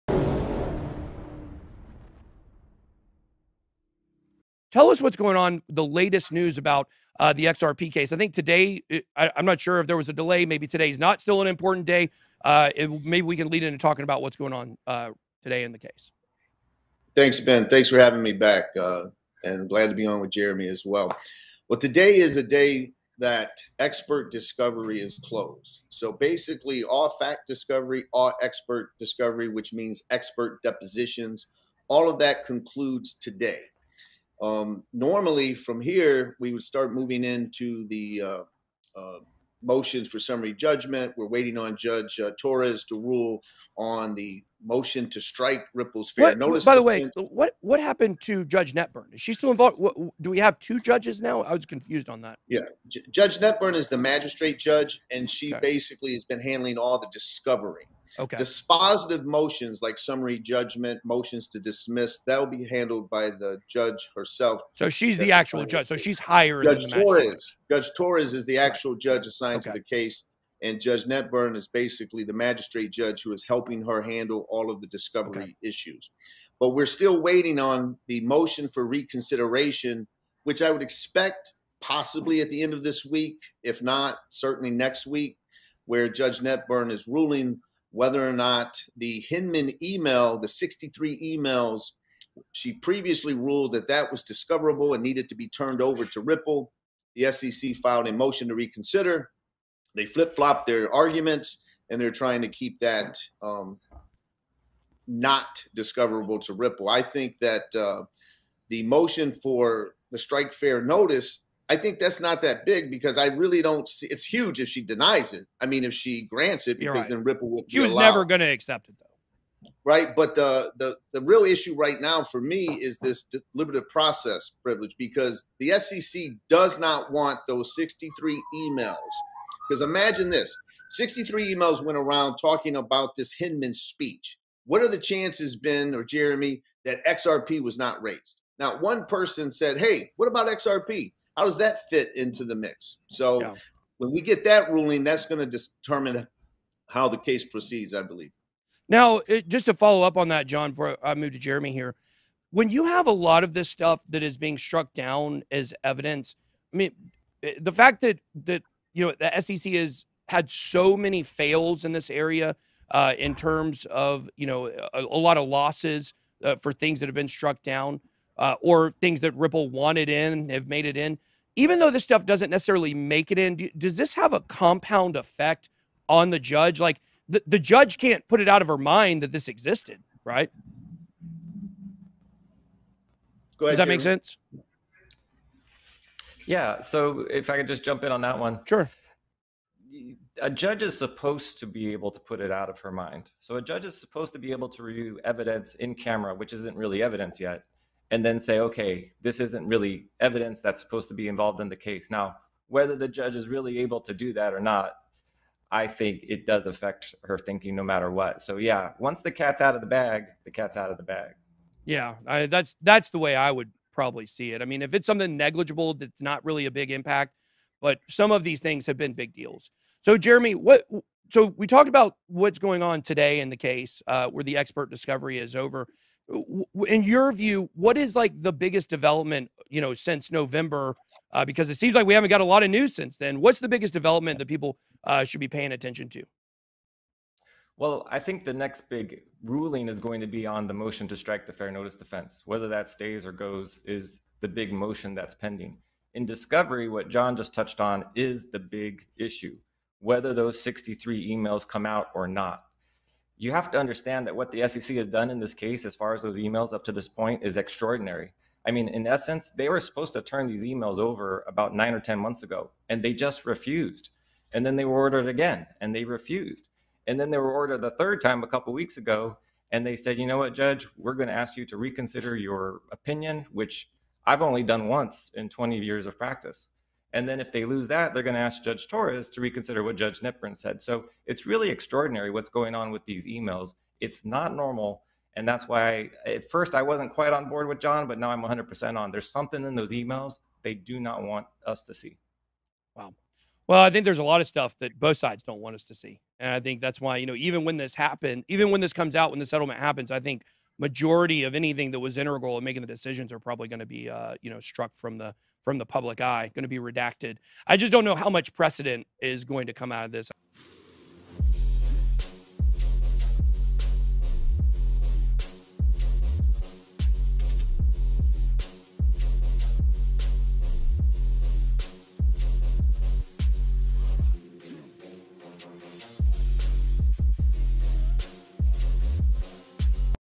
SEC vs Ripple Lawsuit Update [John Deaton and Jeremy Hogan]
Latest updates on the XRP case. Tune in to hear John and Jeremy discuss the details of the proceedings and the next big steps for the case.

All expert discovery is concluding today, however, the motion for reconsideration is still pending, as the SEC does not want certain information to become discoverable.

What they’ve done up to this point is extraordinary, but there’s obviously something they don’t want us to see. How much precedent will be set remains to be seen.